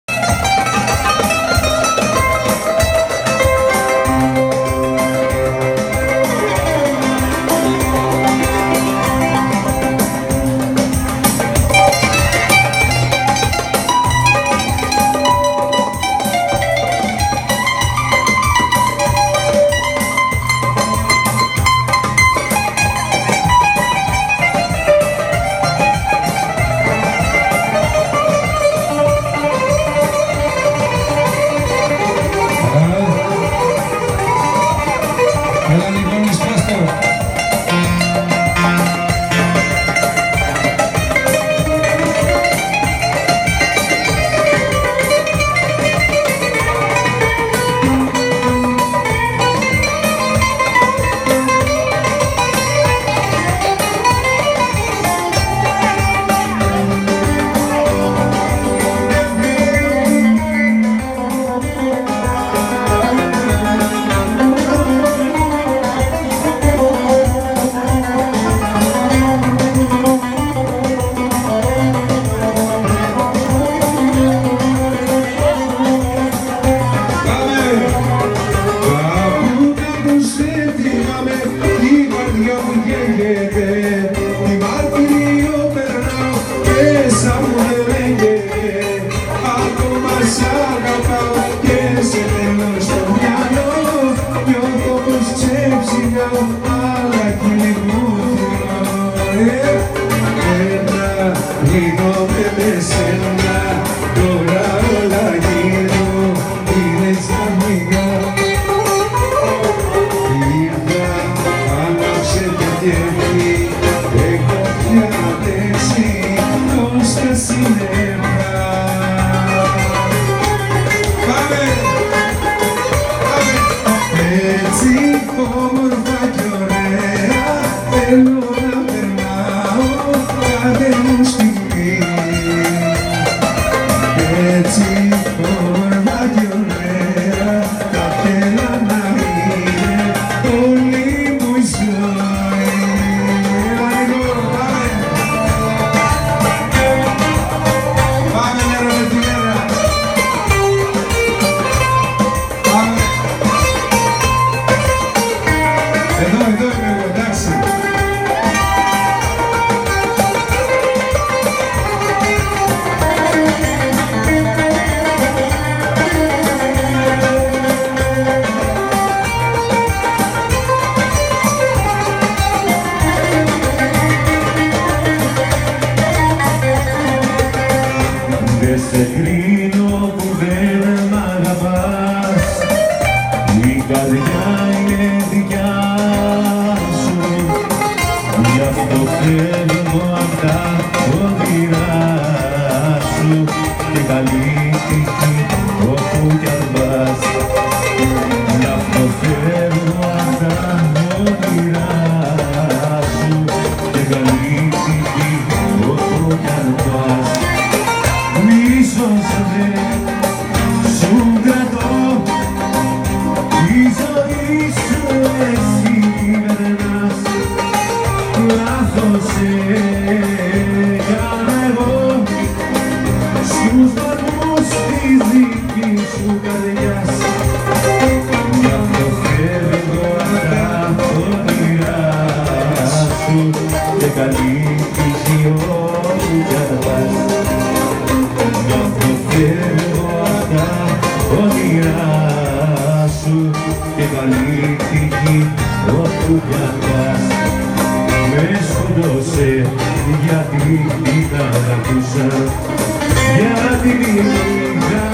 Magazi Baraki New Year Eve 2017 Jim Tsitas & Nikos Mamacus on vocals with Nikos Fostiras on bouzouki
Hope Inn hotel Adelaide
Jerry Tsitas - Entertainer